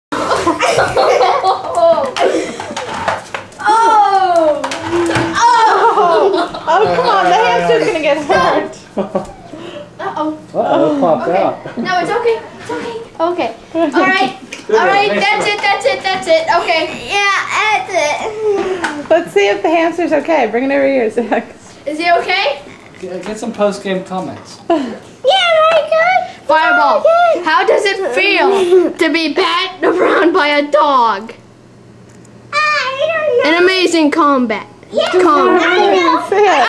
Maci Dog chases Fireball (hamster) Continuation
The conclusion of Maci vs. Fireball...and the confirmation of Fireball's good health for all you animal lovers out there!!